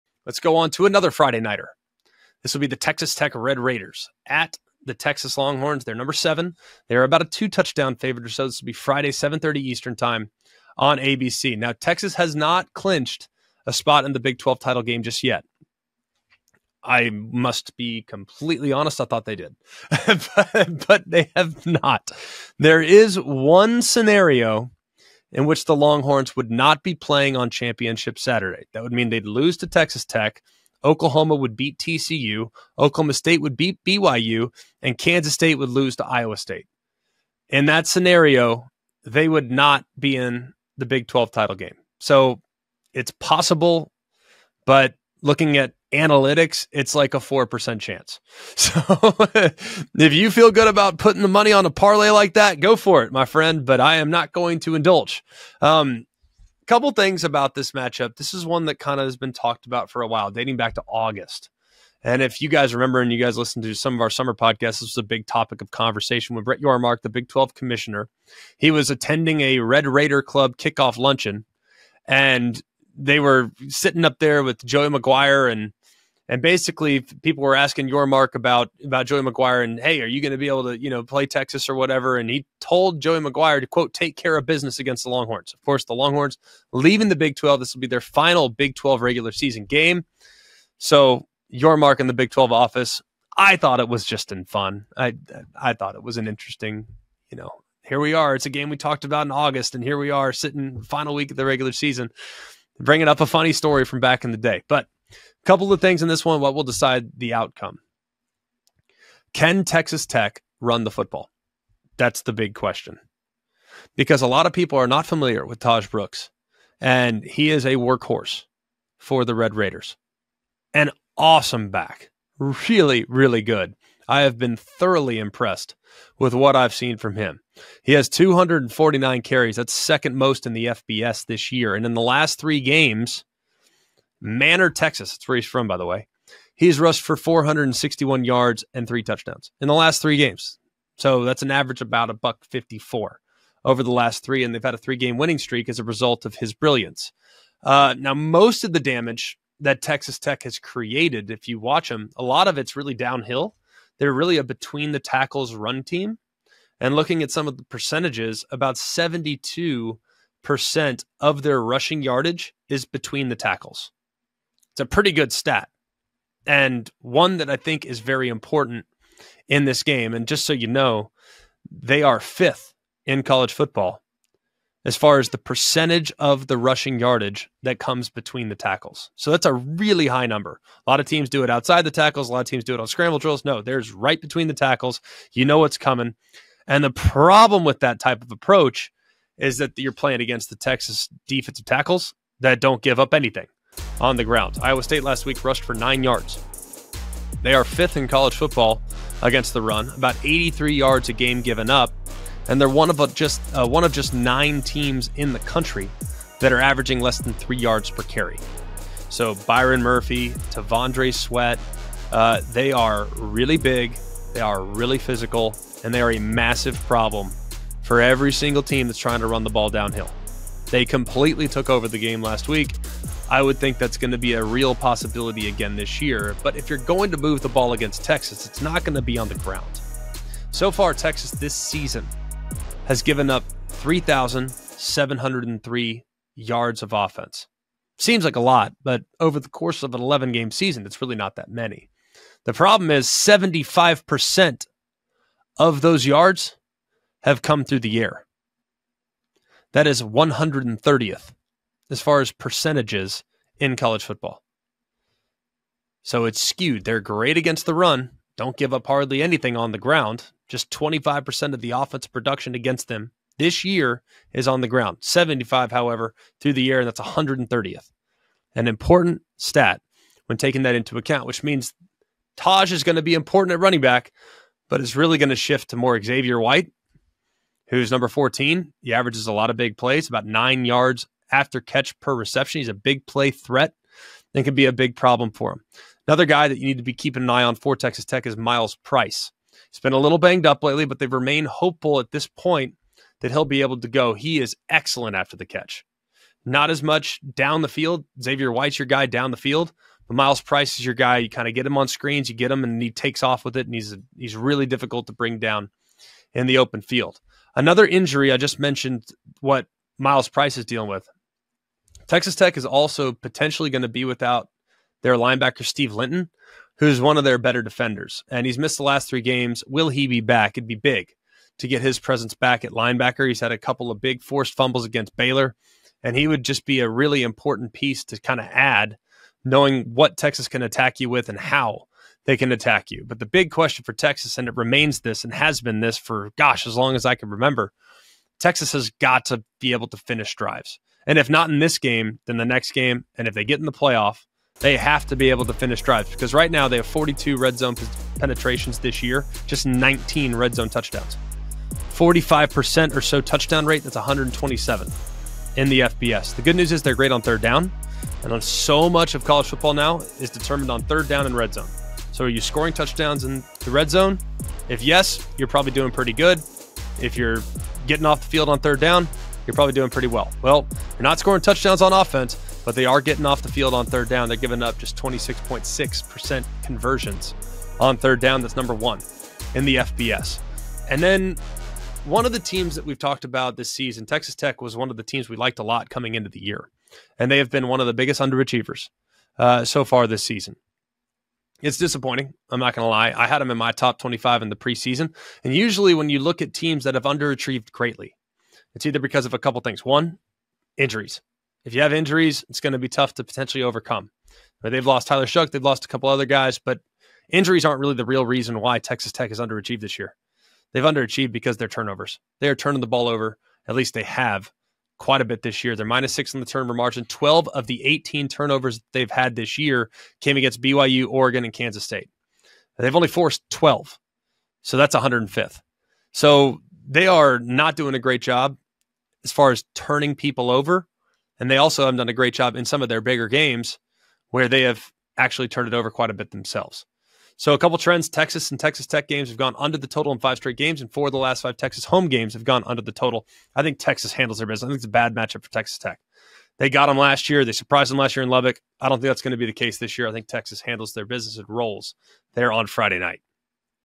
Texas looks to keep CFP hopes alive vs. Texas Tech | Always College Football
On Always College Football, Greg McElroy previews Texas vs. Texas Tech, where Steve Sarkisian hopes to keep the Longhorns’ CFP hopes alive.